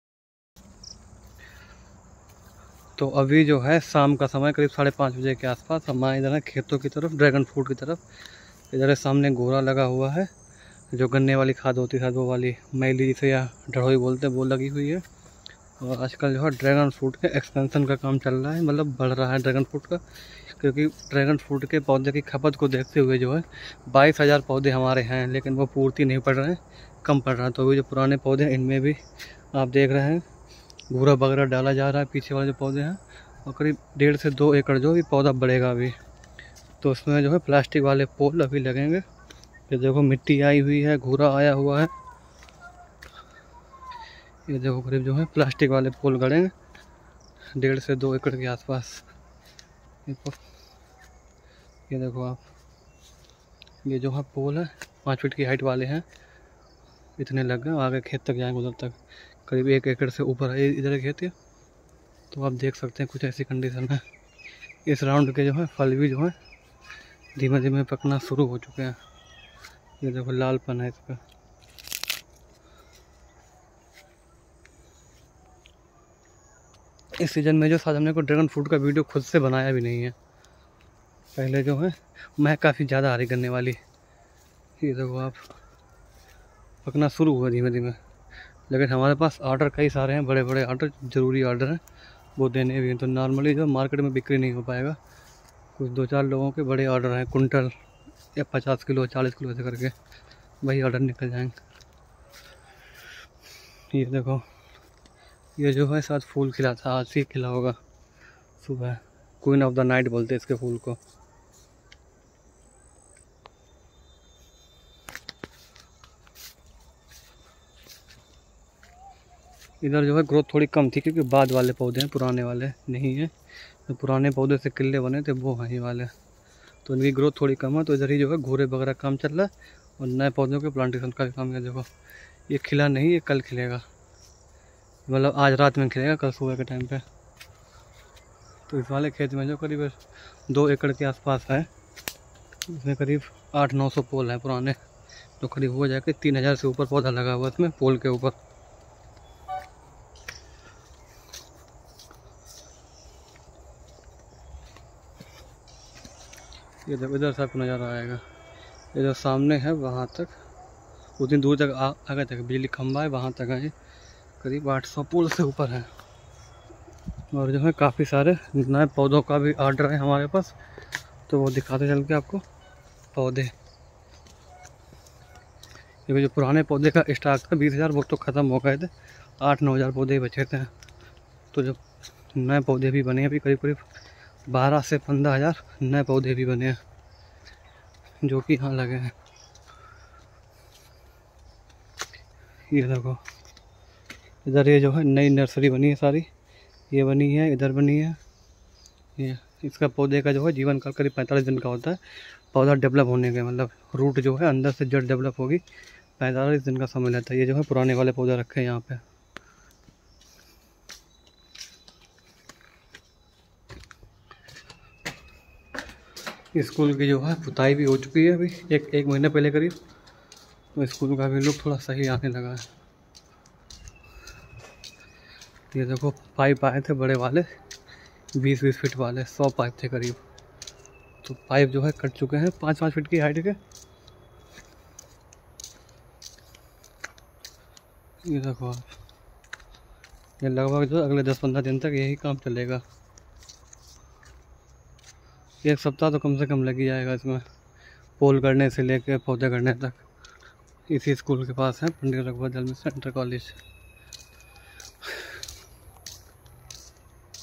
ड्रैगन फ्रूट की भारी मांग को देखते हुए 2 एकड़ में लग रहे नए ड्रैगन फ्रूट के पौधे
Hello Friends, ड्रैगन फ्रूट की भारी मांग को देखते हुए फार्म हाउस पर करीब 2 एकड़ में ड्रैगन फ्रूट के नए पौधे लगाए जा रहे हैं वर्तमान में हमारे पास 22000 पौधे लगे हैं फिर भी मार्केट में ड्रैगन फ्रूट की डिमांड के हिसाब से सप्लाई नहीं हो पा रही है जल्द ही ड्रैगन फ्रूट के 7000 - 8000 नए पौधे लगेंगे